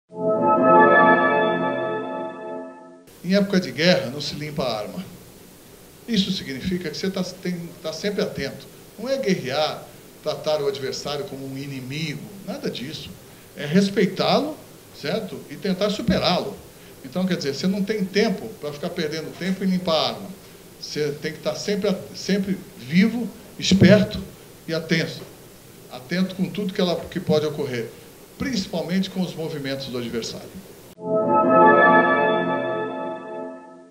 Leão: ' Em época de guerra, não se limpa arma'
Leão compara com 'guerra' jogo contra o Coxa